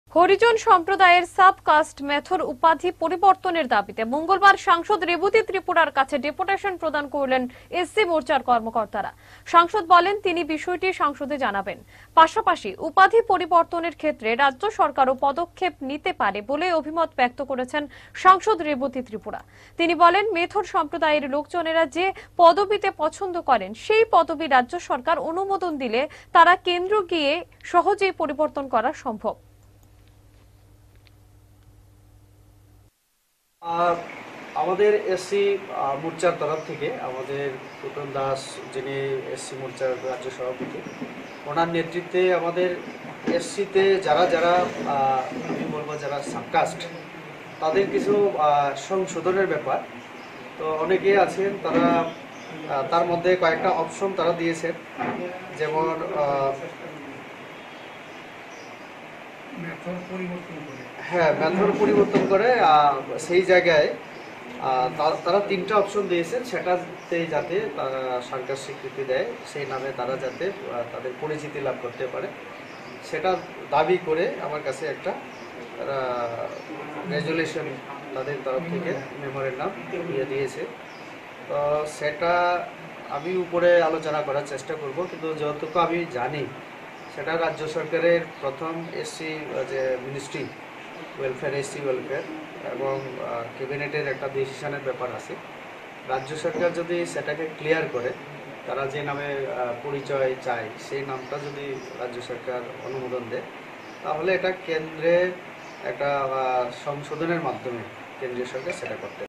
ডেপুটেশন প্রদান করলেন SC মোর্চার কর্মকর্তারা
হরিজন সম্প্রদায়ের সাব-কাস্ট মেথর উপাধি পরিবর্তনের দাবীতে মঙ্গলবার সাংসদ রেবতী ত্রিপুরার কাছে ডেপুটেশন প্রদান করলেন SC মোর্চার কর্মকর্তারা। সাংসদ বলেন, তিনি বিষয়টি সংসদে জানাবেন। পাশাপাশি উপাধি পরিবর্তনের ক্ষেত্রে রাজ্য সরকারও পদক্ষেপ নিতে পারে বলেই অভিমত ব্যক্ত করেছেন সাংসদ রেবতী ত্রিপুরা। তিনি বলেন, মেথর সম্প্রদায়ের লোকজনেরা যে পদবীতে পছন্দ করেন সেই পদবি রাজ্য সরকার অনুমোদন দিলে তারা কেন্দ্রে গিয়ে সহজেই পরিবর্তন করা সম্ভব।